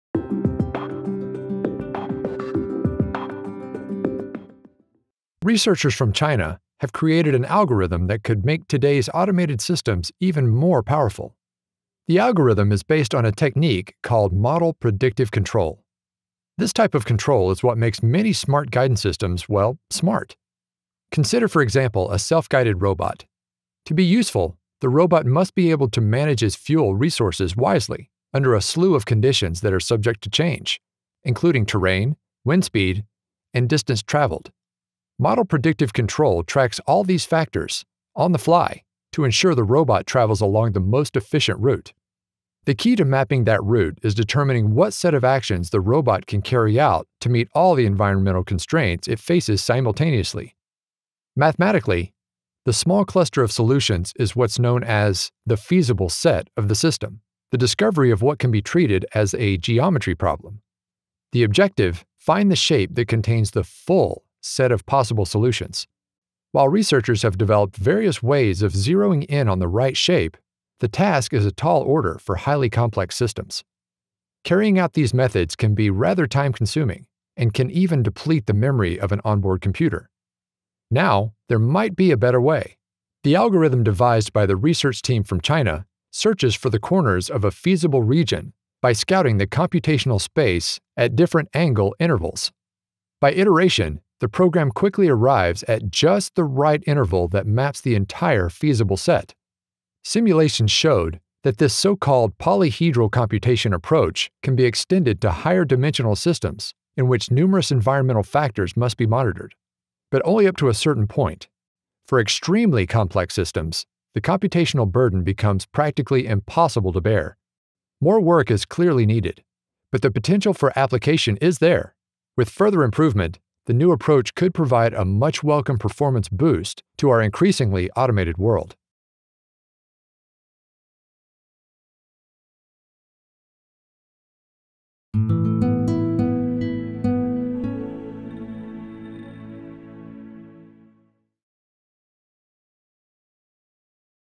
New algorithm could improve automated systems
Xie et al. "Polyhedral Feasible Set Computation of MPC-Based Optimal Control Problems." IEEE/CAA Journal of Automatica Sinica (2018). doi: 10.1109/JAS.2018.7511126